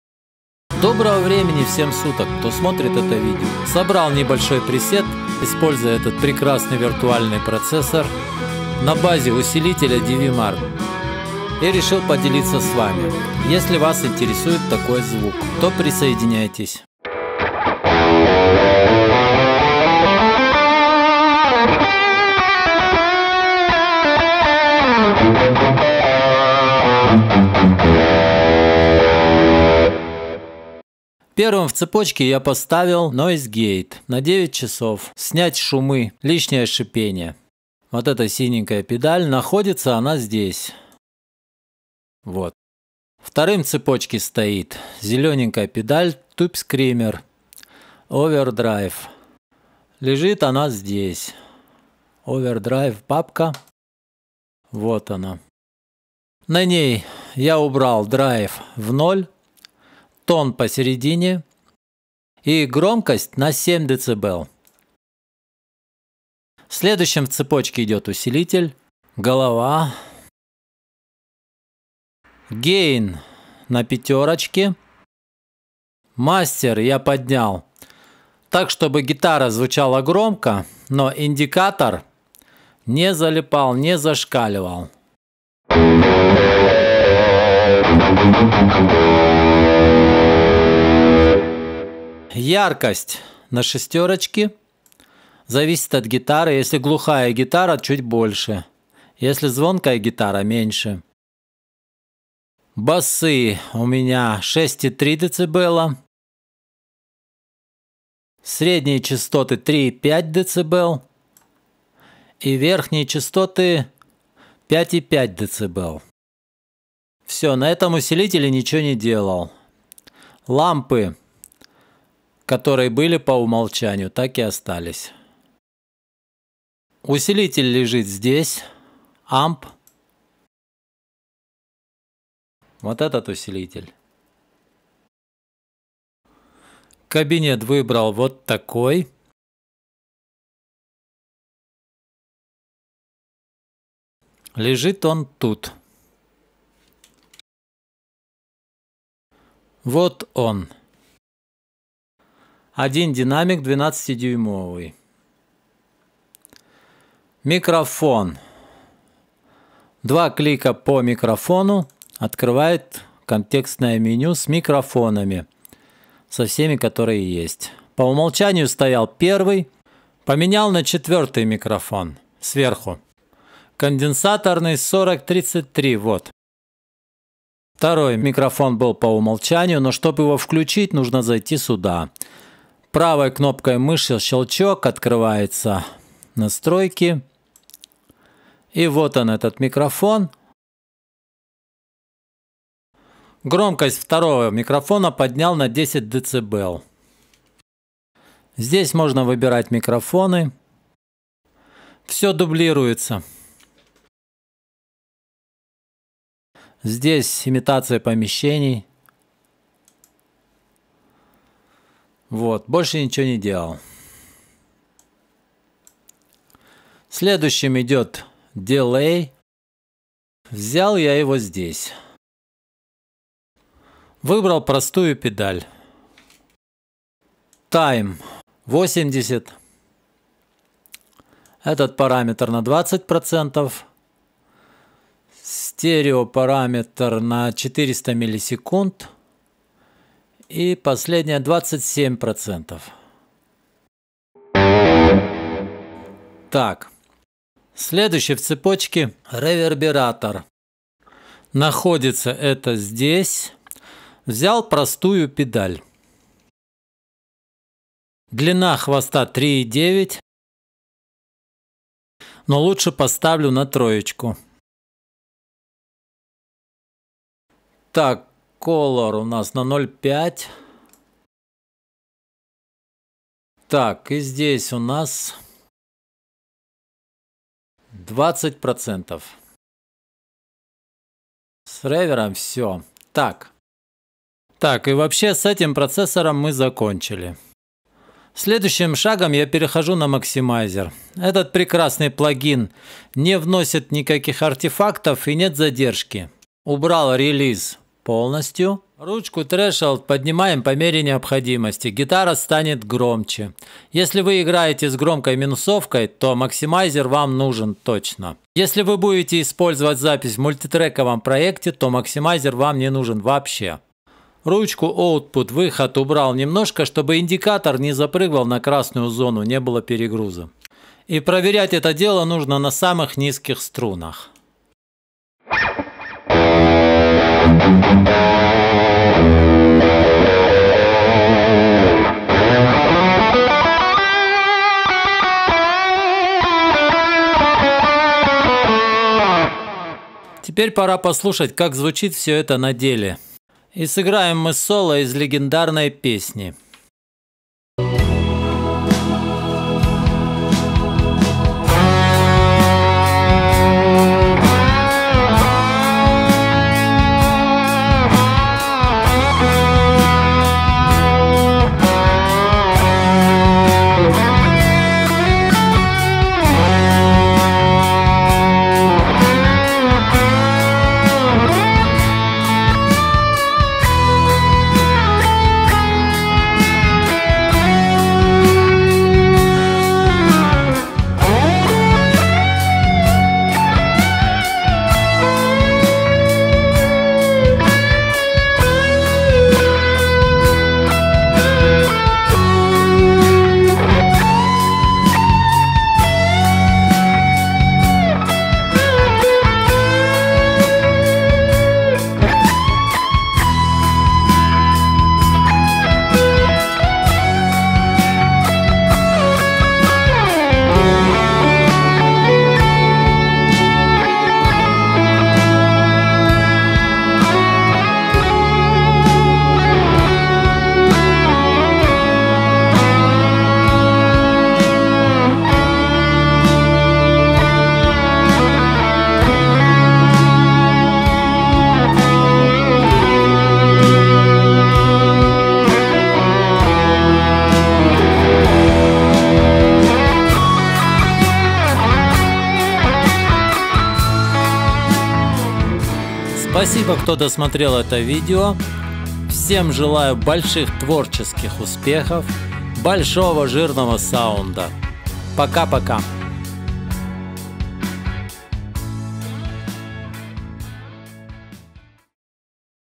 Overloud TH U {DV Mark} Как создать современный жирный звук для Соло гитары
Помощь каналу;  СберБанк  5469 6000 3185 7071